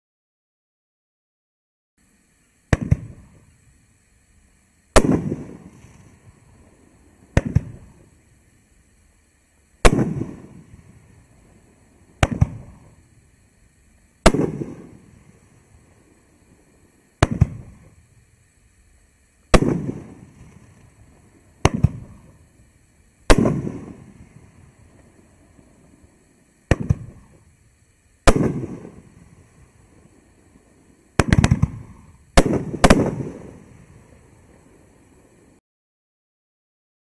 Tropical Vibes | 9-Shot NOAB by Boom Wow
Red Coco Silver Plum, Green Coco Red Plum, Purple Coco Yellow Plum, With Whirlwind
Flower Red Strobe Finale. 3"' 9 Shots, 500-gram NOAB by Boom Wow Fireworks, Available at the Iowa Fireworks Farm.